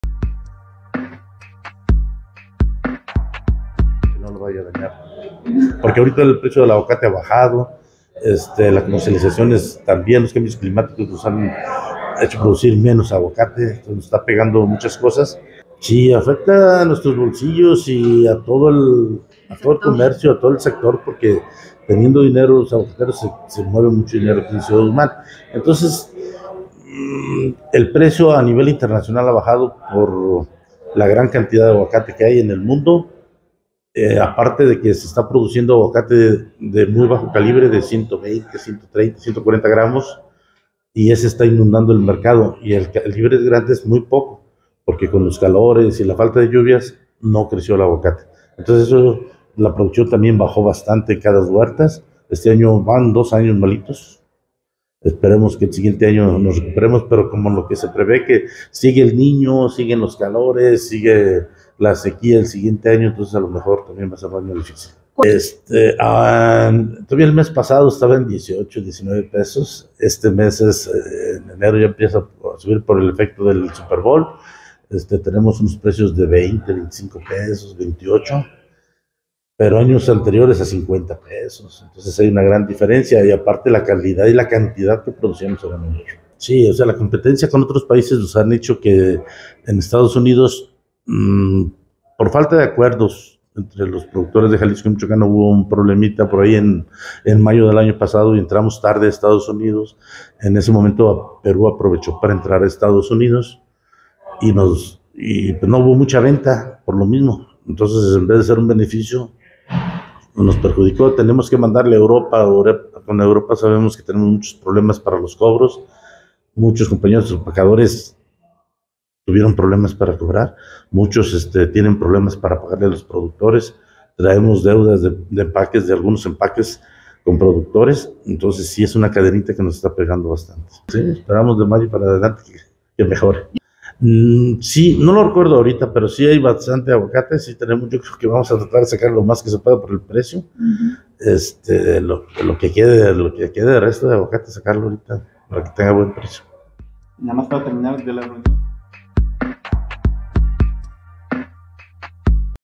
A la baja precio del aguacate
En las últimas semanas ha bajado el precio del aguacate, pero esperan los productores que con el Super Bowl mejore no solo el precio, sino las ventas en Estados Unidos.